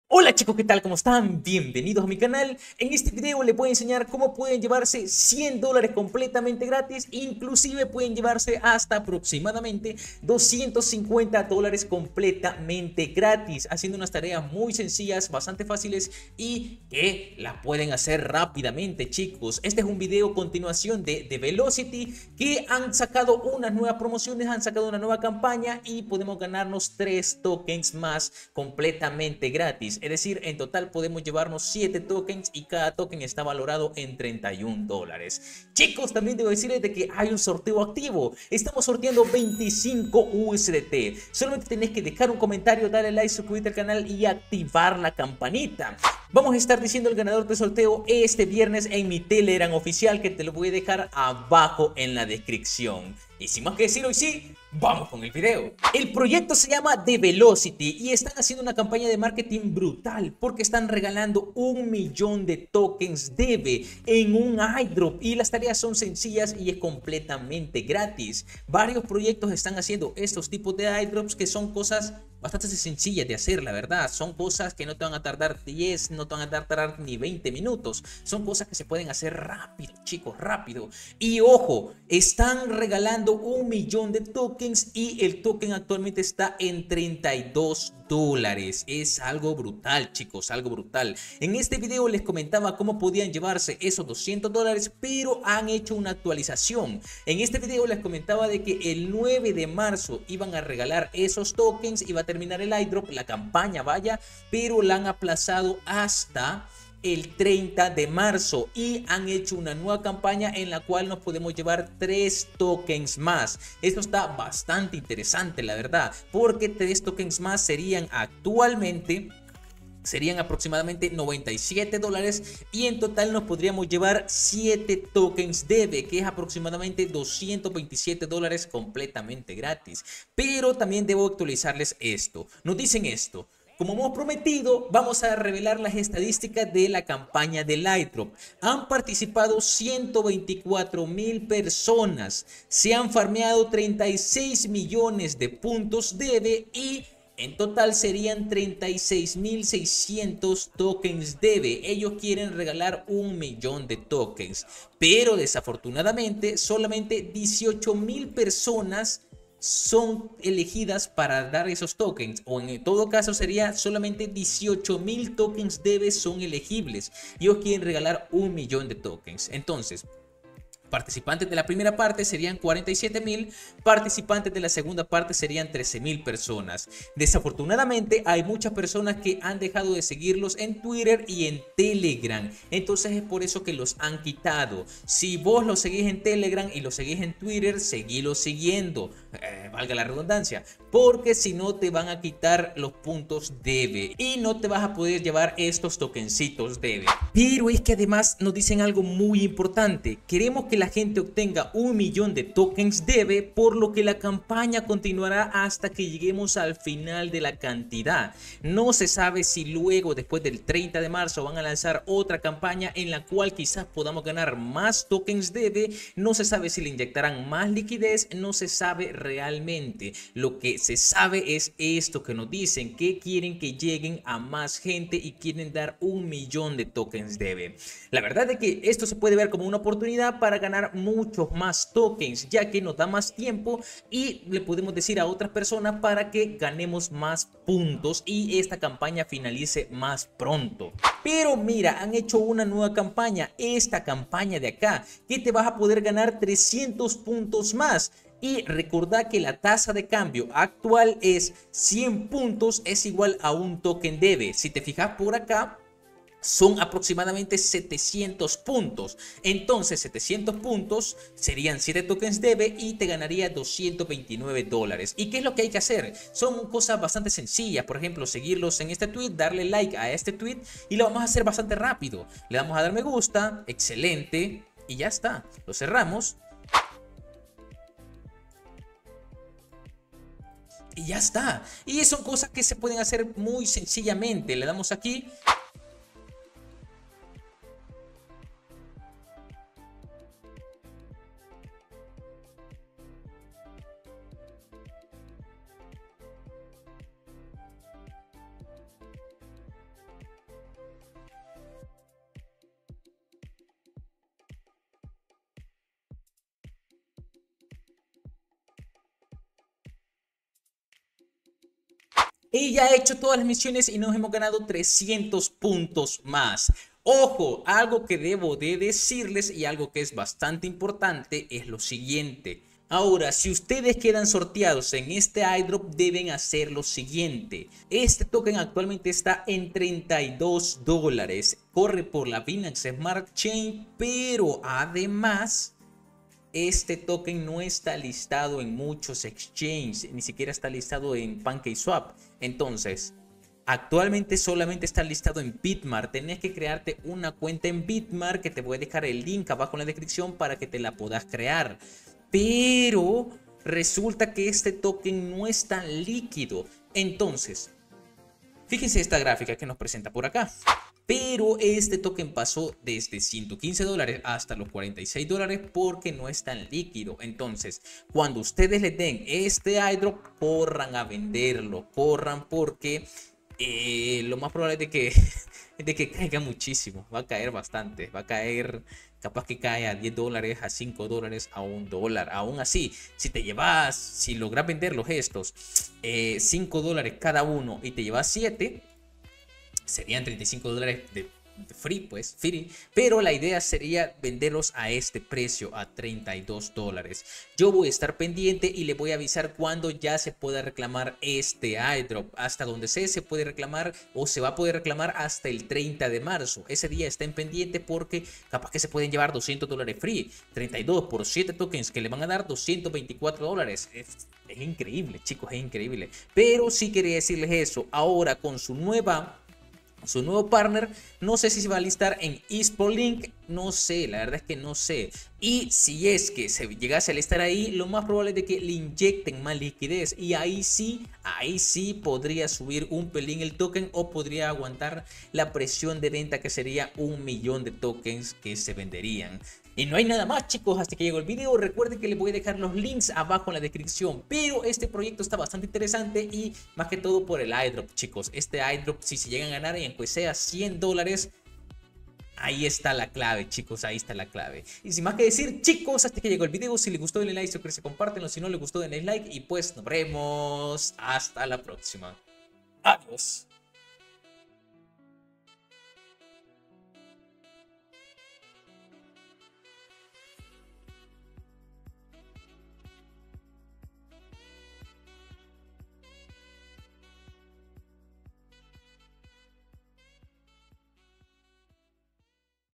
💥GANA 250 DOLARES GRATIS SIN INVERSION CON LA NUEVA CAMPAÑA DE DEVE AIRDROP💥
En este video te presento una forma en la que puedes GANAR DINERO COMPLETAMENTE GRATIS! 250 DOLARES GRATIS PARA TI! PARA TODOS! CORRE!

ESTA NO ES RECOMENDACION DE COMPRA NI DE VENTA! HAZ TU PROPIO ANALISIS!